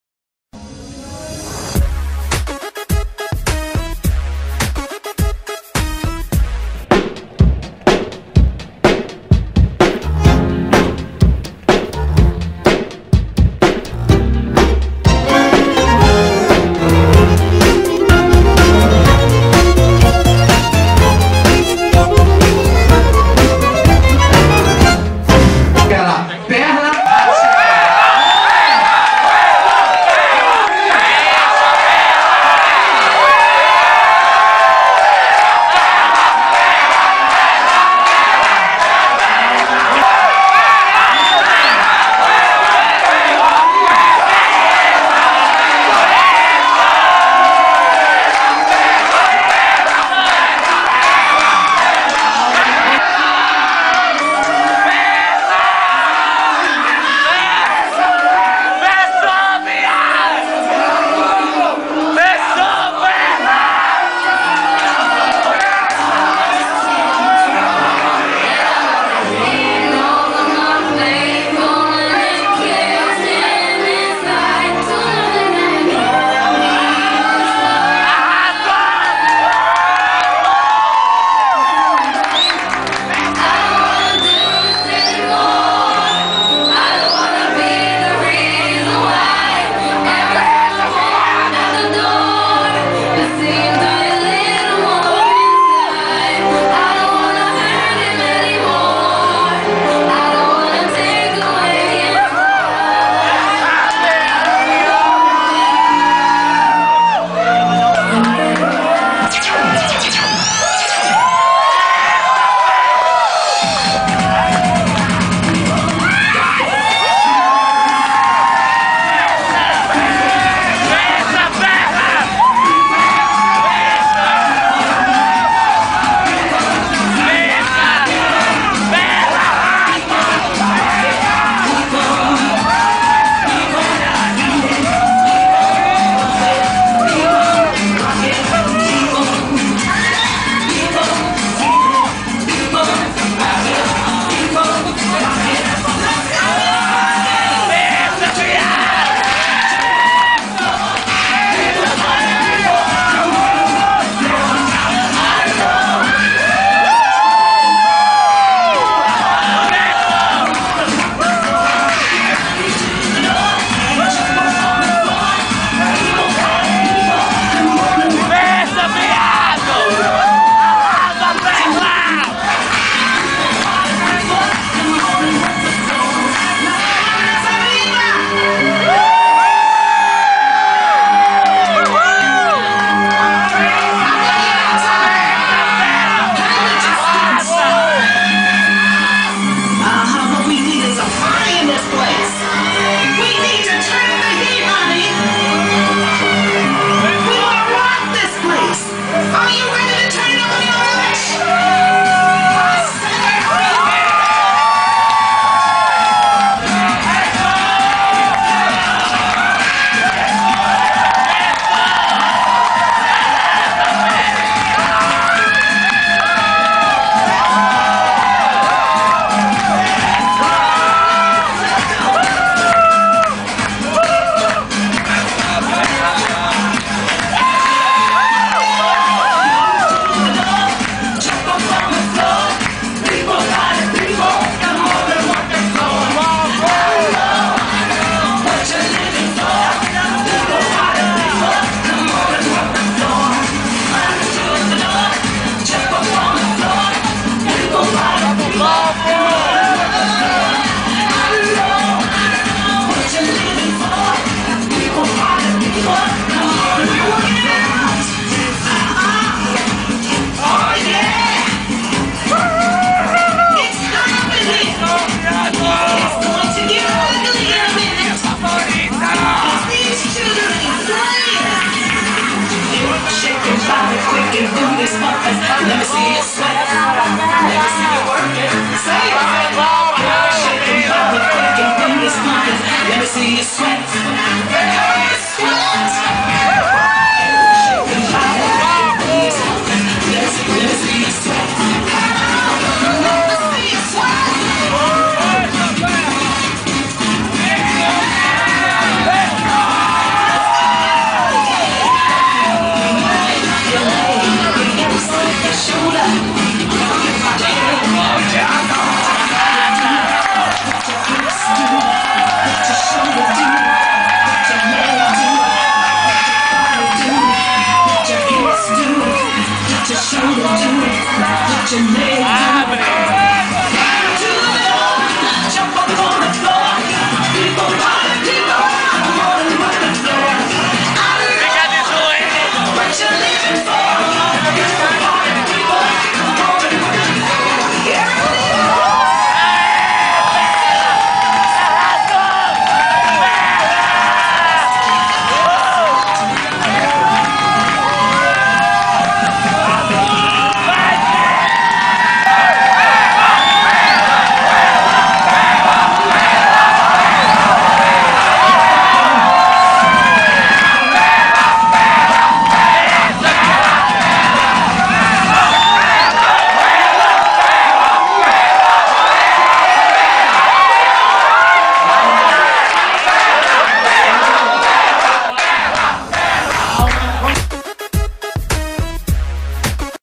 Perlla Rachelly - Final do concurso A Favorita Boate Vogue JP
Perlla Rachelly com sua originalidade e criatividade , ovassionada pela galera da vogue jp já entra emocionada e leva a galera ao delirio com sua performance sobre os patins, "bater cabelo já é dificil imagina de patins" , ela simplesmente arrazou!!!